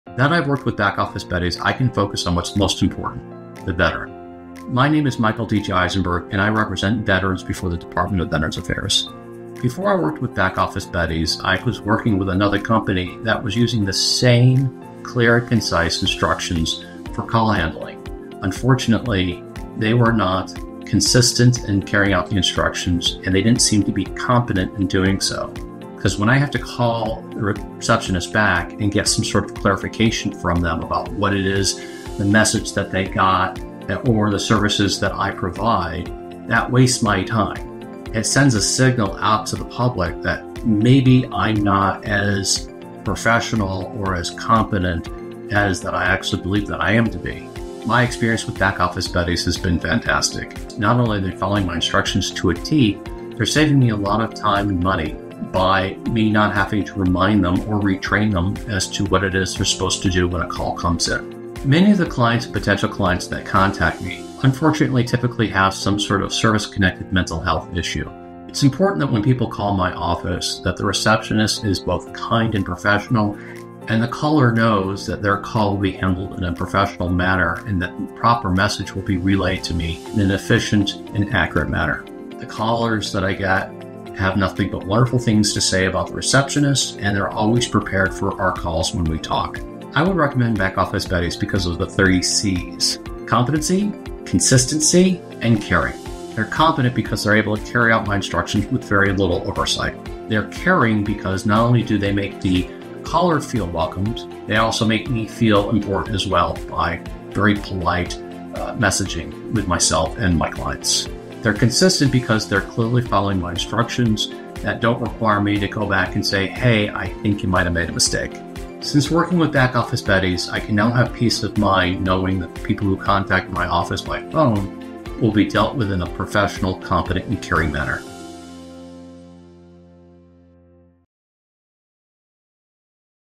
3 C's of Success: Michael's Veteran Advocacy Journey
Meet Michael, an attorney dedicated to advocating for veterans. With Back Office Betties, he discovered the 3 C's: Confident, Consistent & Caring service. Since partnering with Betties, Michael can now focus on what matters most: helping veterans. Before, inconsistent reception services cost him time and reputation. Now, with Back Office Betties, his firm's procedures are flawlessly executed, giving him peace of mind and the freedom to make a difference.

Learn how Back Office Betties can support your mission!

for more details.